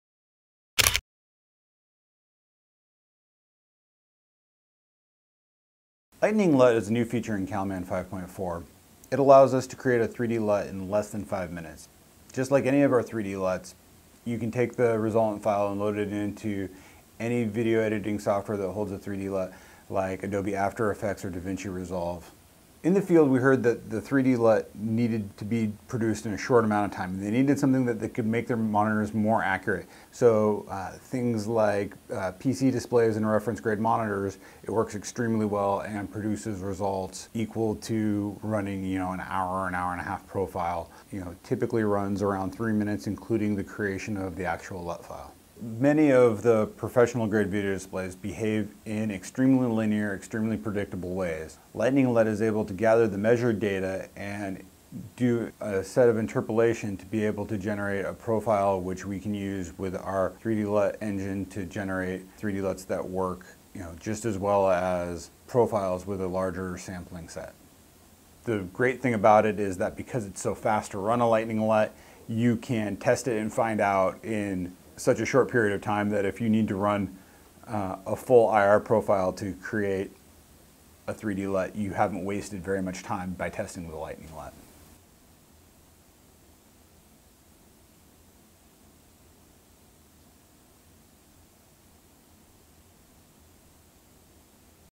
CalMAN Lightning LUT creates corrective LUT in under 5 minutes
SpectraCal’s Lead Software Developer Joel Barsotti discusses the new Lightning LUT technology in CalMAN 5.4.0. With Lightning LUT, creative professionals can correct the color output of their reference video monitors in five minutes or less. Unique interpolation algorithms allow CalMAN to create precise correction data with less than 80 reads. CalMAN Lightning LUTs can be loaded into any finishing software or supported hardware device. Lightning LUT technology is included in CalMAN Studio, CalMAN Studio Lite and CalMAN Studio Express.